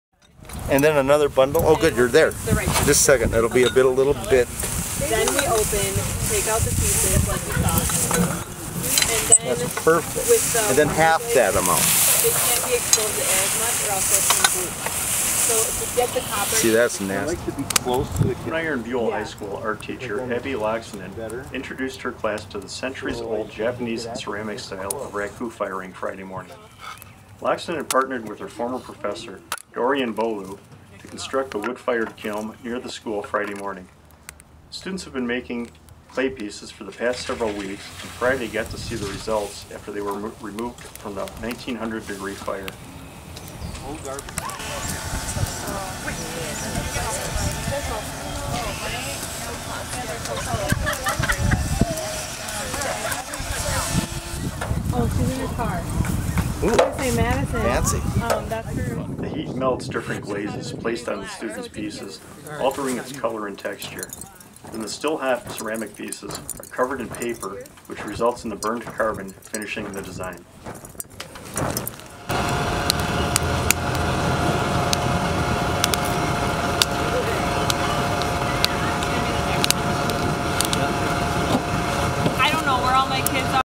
Making Art
Mountain Iron-Buhl High School students learn about Raku Firing Friday morning.